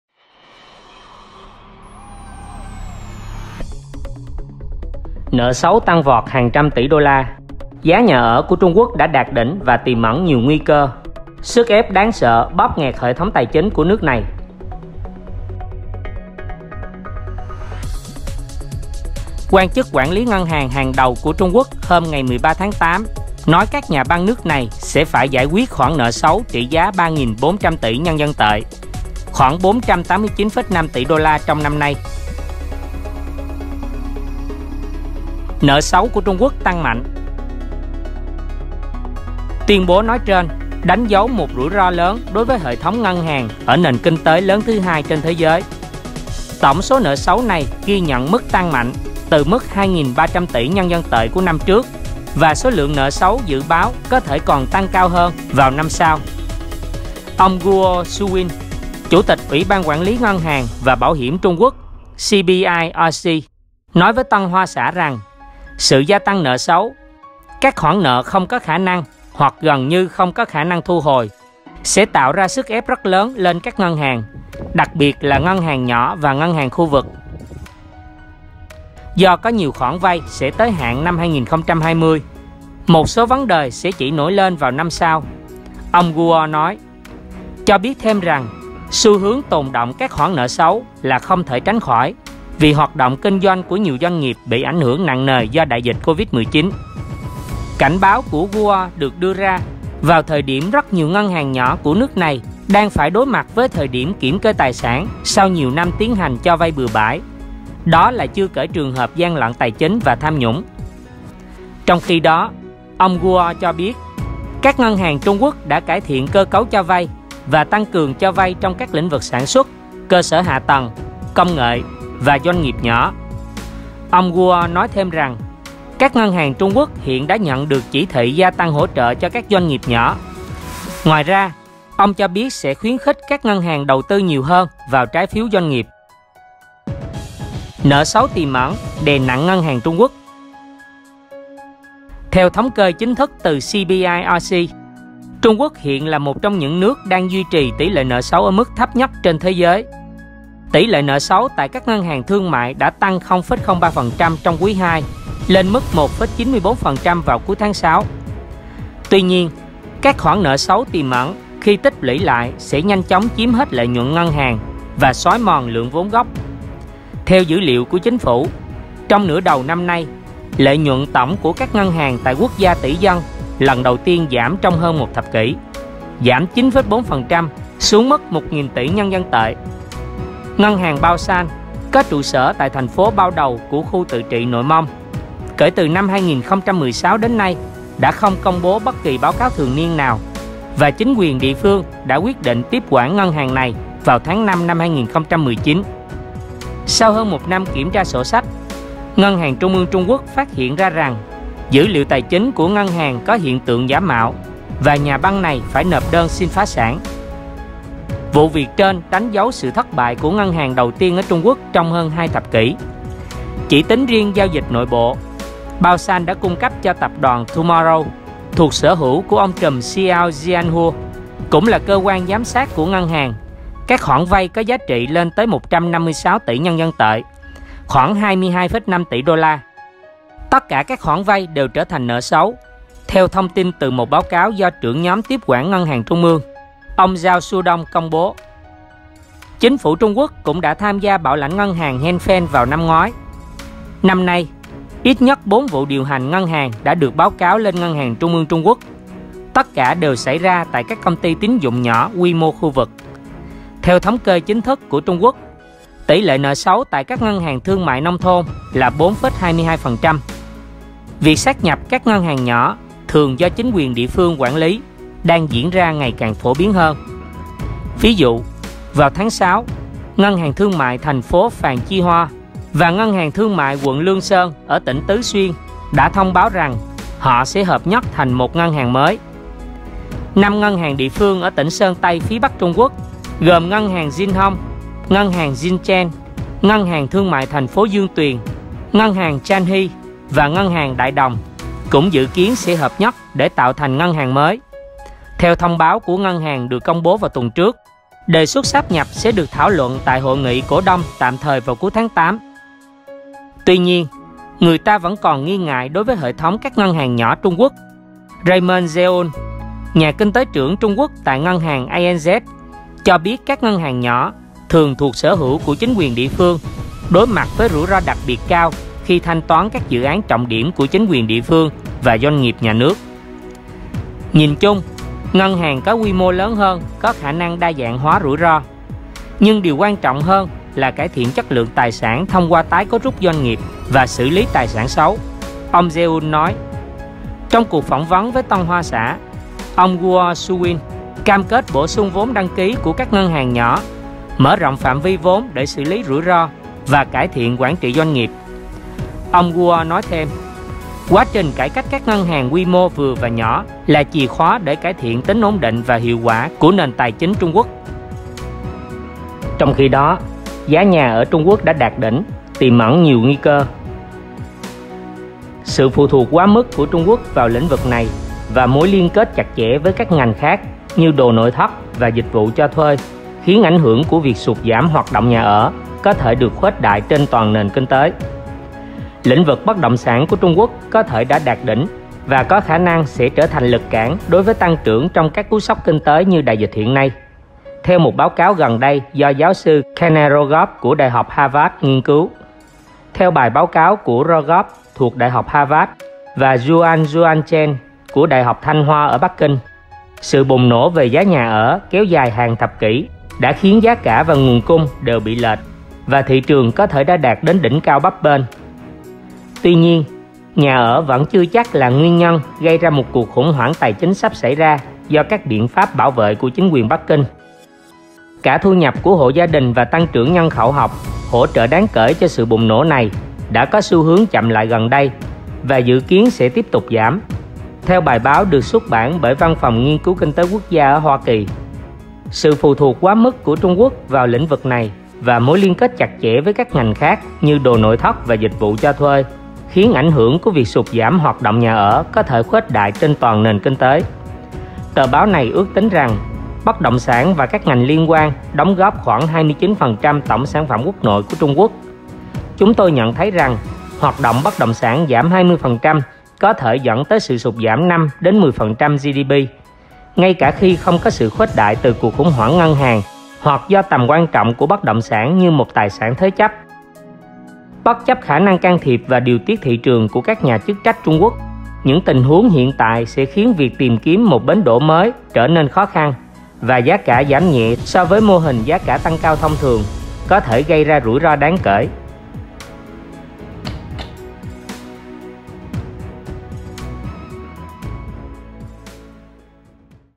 Nợ Xấu Tăng Vọt vs Giá Nhà Đạt Đỉnh - Sức Ép Đáng Sợ Lên Hệ Thống Tài Chính Trung Quốc | DVS Vlog
Trung Quốc thừa nhận nợ xấu tăng vọt hàng trăm tỉ USD: Sức ép đáng sợ "bóp nghẹt" hệ thống tài chính?

Quan chức quản lý ngân hàng hàng đầu của Trung Quốc hôm 13/8 nói các nhà băng nước này sẽ phải giải quyết khoản nợ xấu trị giá 3.400 tỷ NDT (489,5 tỷ USD) trong năm nay.

- 0:34 Nợ xấu của Trung Quốc tăng mạnh
- 2:16 Nợ xấu tiềm ẩn đè nặng ngân hàng Trung Quốc
- 5:58 Giá nhà ở Trung Quốc đã đạt đỉnh, tiềm ẩn nhiều nguy cơ